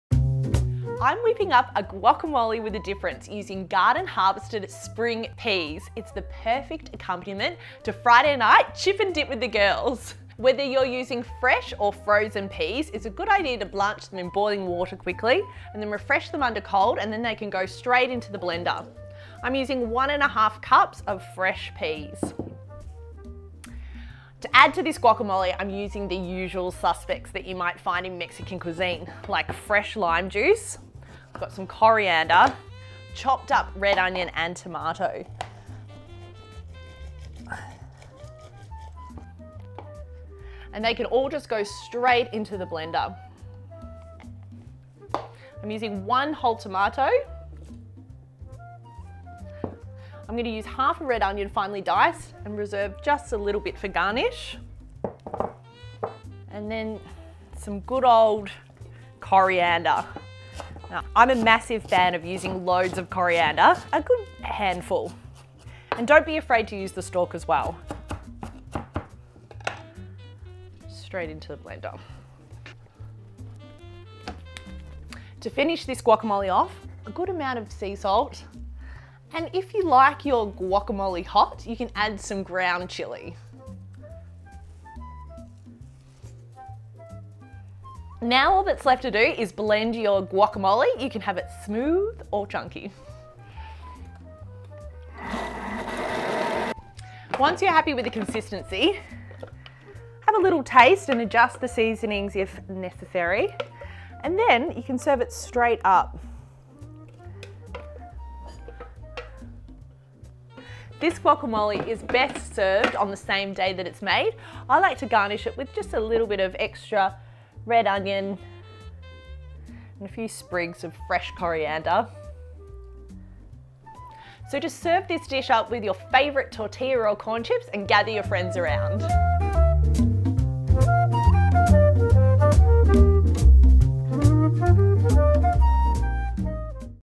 Pea Guacamole // Pea Dip by Dani Venn // Vegan Dip // The Gardenettes
Dani uses peas instead of avocado to make a delicious guacamole style dip. You can use fresh or frozen peas to create this dip and it's the perfect kinda health dip to go with evening drinks or weekend lunch time BBQ.

Recipe created by Dani Venn.